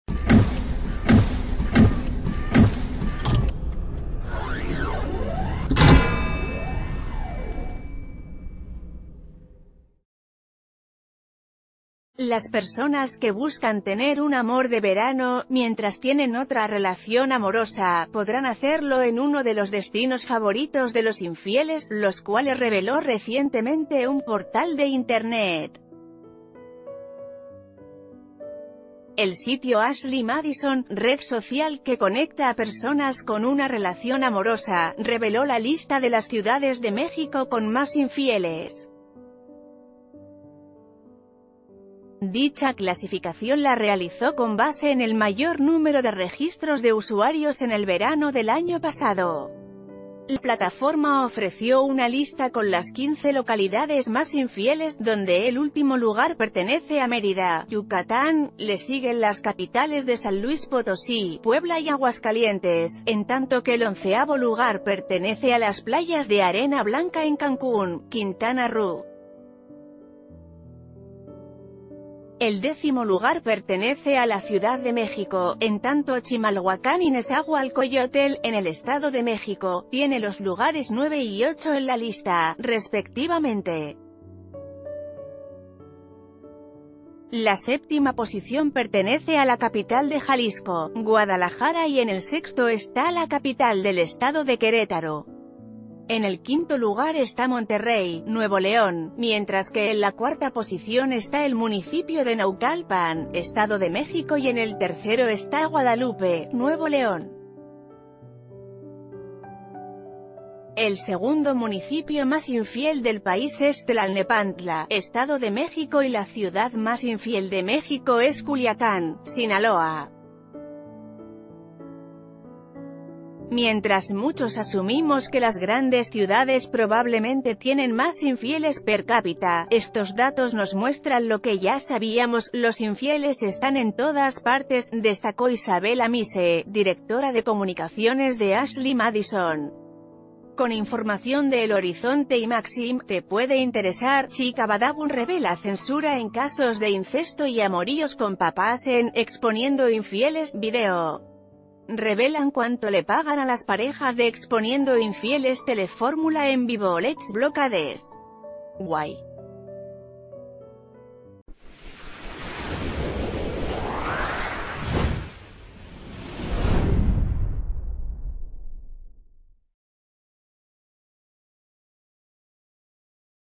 Culiacán es la ciudad con más infieles durante el verano en México
Las personas que buscan tener un amor de verano, mientras tienen otra relación amorosa, podrán hacer...